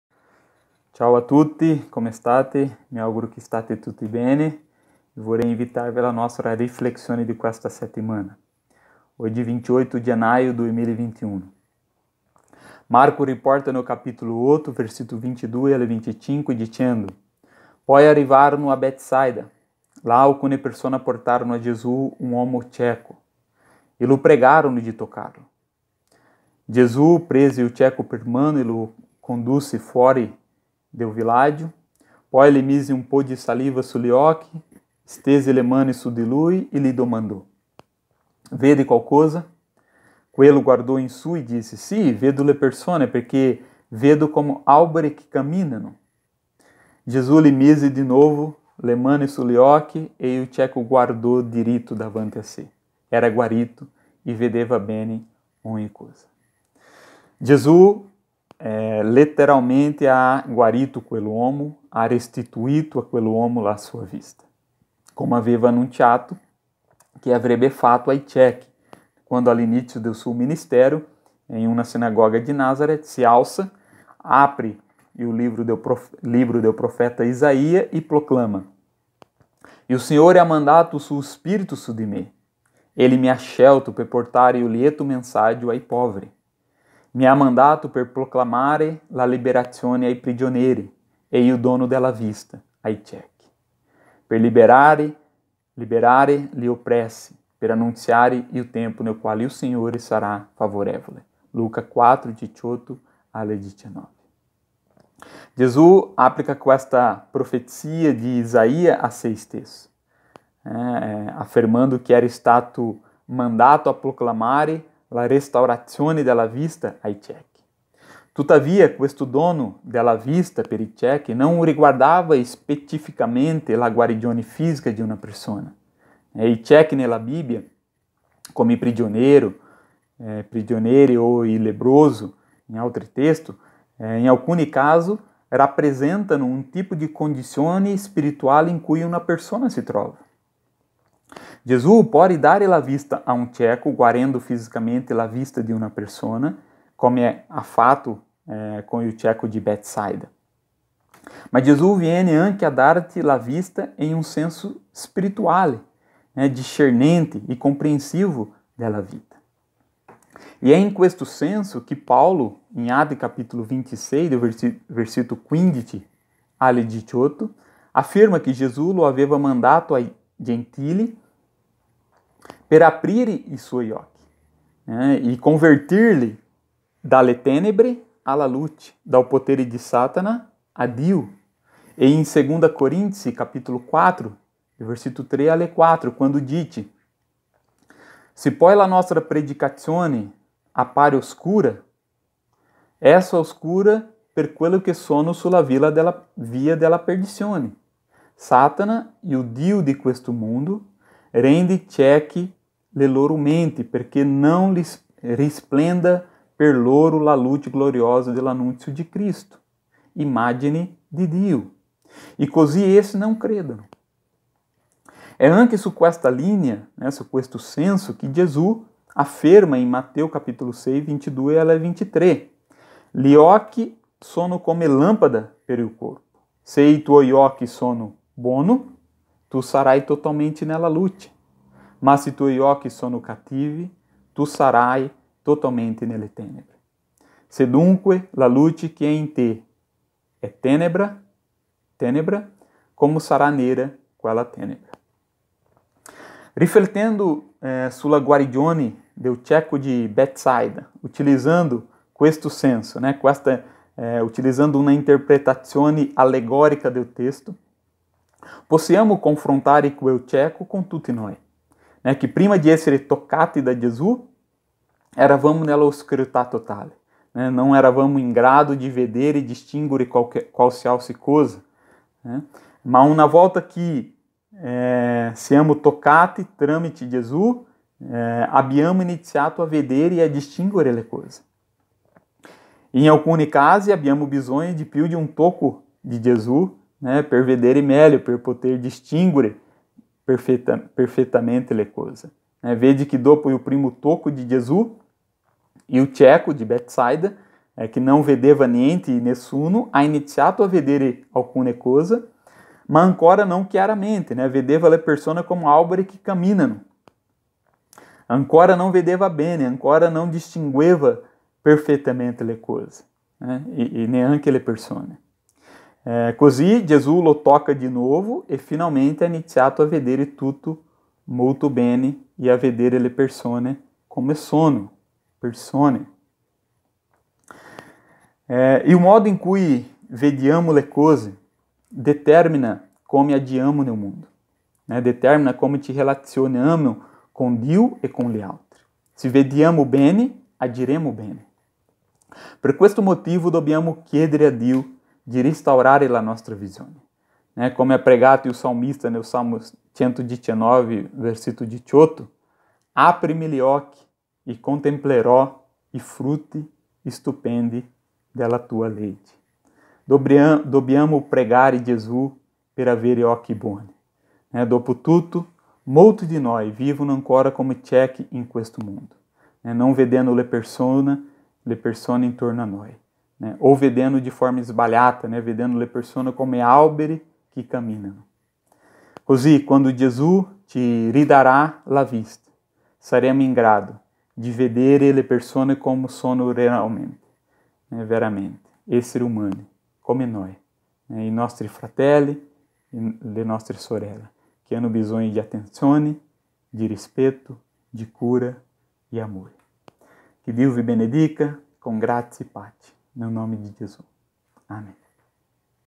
Vedi bene: gli alberi non camminano!
"Poi arrivarono a Betsàida. Là alcune persone portarono a Gesù un uomo cieco e lo pregarono di toccarlo. Gesù prese il cieco per mano e lo condusse fuori del villaggio. Poi gli mise un po’ di saliva sugli occhi, stese le mani su di lui e gli domandò: - Vedi qualcosa? Quello guardò in su e disse: - Sì, vedo le persone; perché vedo come alberi che camminano. Gesù gli mise di nuovo le mani sugli occhi, e il cieco guardò diritto davanti a sé: era guarito e vedeva bene ogni cosa.” (Marco 8.22-25)